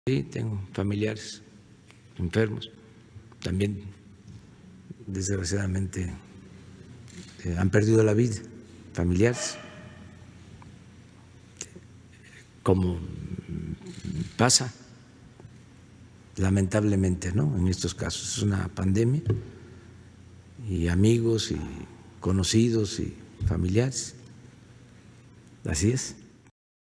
confiesa que FAMILIARES suyos han muerto de COVID19
El presidente Andrés Manuel López Obrador dio a conocer que algunos de sus familiares se han infectado con coronavirus y que incluso alguno de ellos ha muerto por la enfermedad.